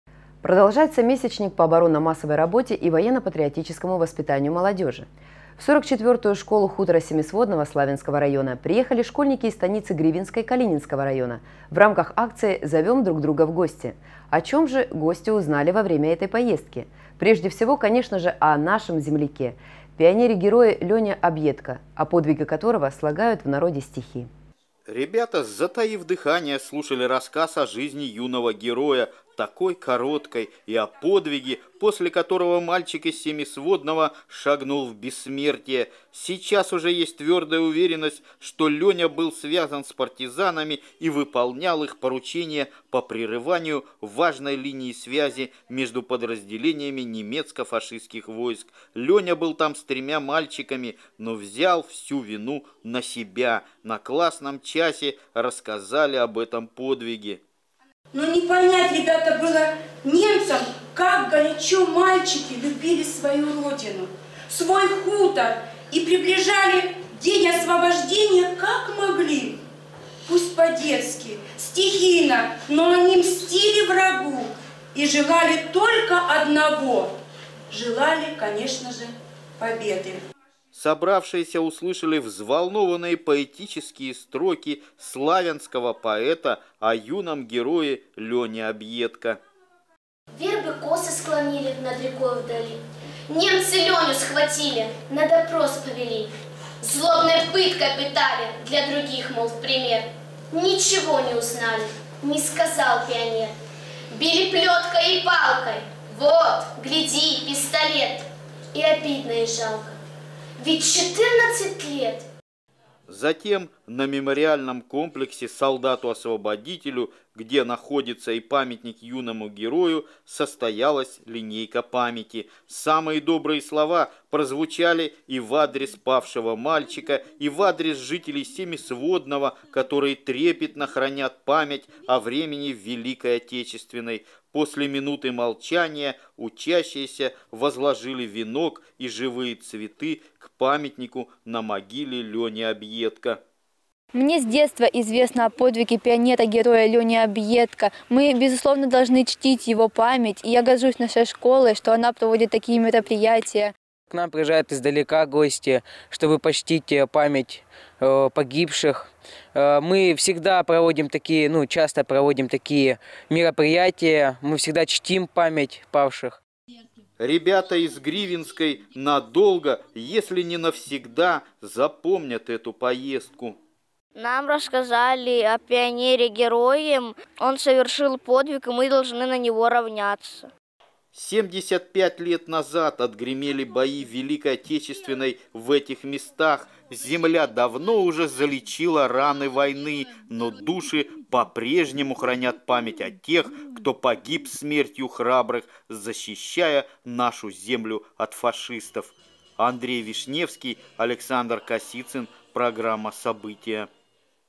Акция «Зовем друг друга в гости»
Продолжается месячник по оборонно-массовой работе и военно-патриотическому воспитанию молодежи.  В сорок четвертую школу хутора Семисводного Славянского района приехали школьники из  станицы Гривенской Калининского района в рамках акции «Зовем друг друга в гости». О чем же гости узнали во время этой поездки? Прежде всего, конечно же, о нашем земляке, пионере-герое Лене Объедко, о подвиге которого слагают в народе стихи.